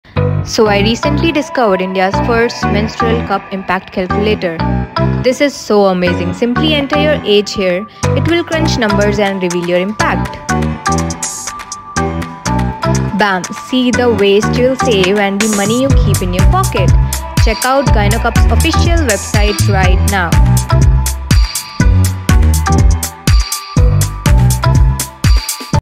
India's first Menstrual Impact Calculator by GynoCup | Mildcares
How much does a period cost you on average in your lifetime?

GynoCup is proudly made in India

You’ve probably heard that switching to the GynoCup® saves you money. But have you ever calculated how much money you could save or thought about what you could spend that money on?

Each one of us is estimated to have approximately 500 periods during our lifetime. That’s 6,5 years we spend menstruating and using an average of 11,000 disposable period products, many of which end up in landfills or worse – in our oceans.

You only need one GynoCup, regardless of your flow. And one menstrual cup will last you years and years. All the while you’ll be saving the planet, one period at a time.

We’re so sure you’ll love your GynoCup® that we’re offering all our customers a satisfaction guarantee. Why not calculate how much you could save by switching to a GynoCup menstrual cup?